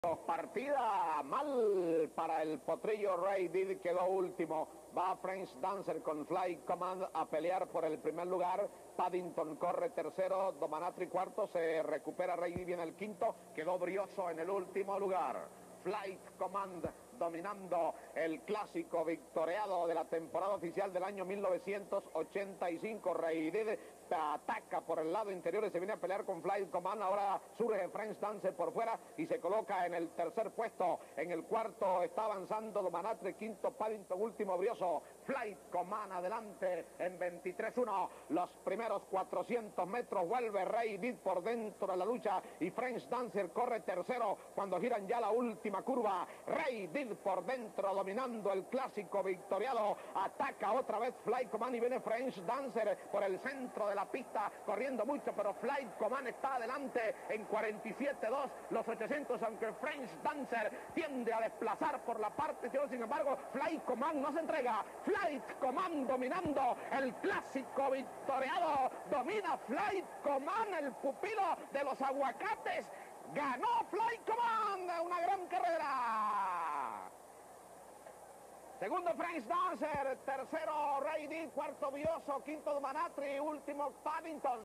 🔆 Clasico Victoreado 1985 FLIGHT COMMAND 🔆
Partidaaaa ! Mal para Rey Did, a la delantera sale French Dancer con Flight Command a luchar por el primer lugar, lo siguen: Paddington, Domanatri, Rey Did se queda Brioso en el ultimo lugar. Estan girando la ultima curva Rey Did esta dominando la prueba ataca de nuevo Flight Command y pasa a dominar French Dancer lo acecha pero Flight Command no puede perder, gano Flight Command 2do French Dancer, 3ro Rey Did, 4to Brioso 5tp Domanatri y ultimo Paddington.

Jinete: Argimiro Guerrero
Entrena: V. Urbina
Narra: Aly Khan
Haras: Los Aguacates